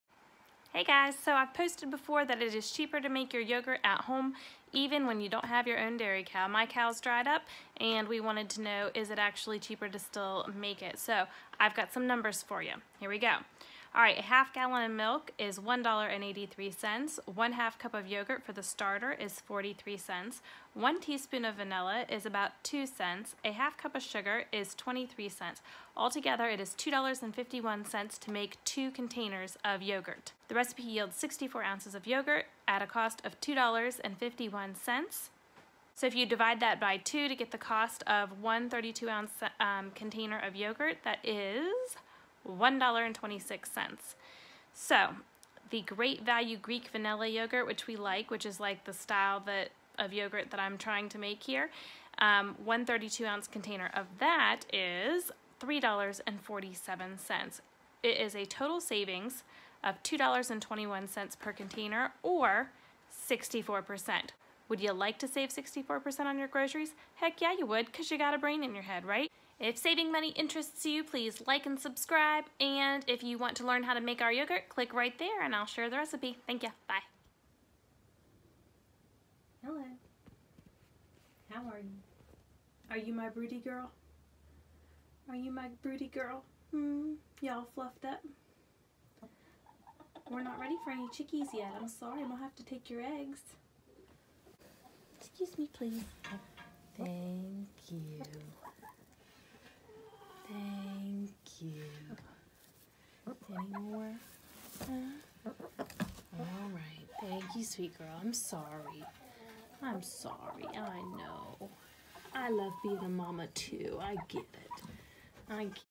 Frugal win? Is it cheaper to make your own yogurt? Real numbers!
If you like saving money on groceries, than this video is for you.  Learn the exact cost of the ingredients I use in my yogurt and just how much can be saved! :)

Products you may like:
Yogurt starter if you would like to have something shelf-stable
Fancy slow cooker for making yogurt (among other things)
InstantPot for making yogurt (among other things)
Lower-fuss yogurt maker
My shirt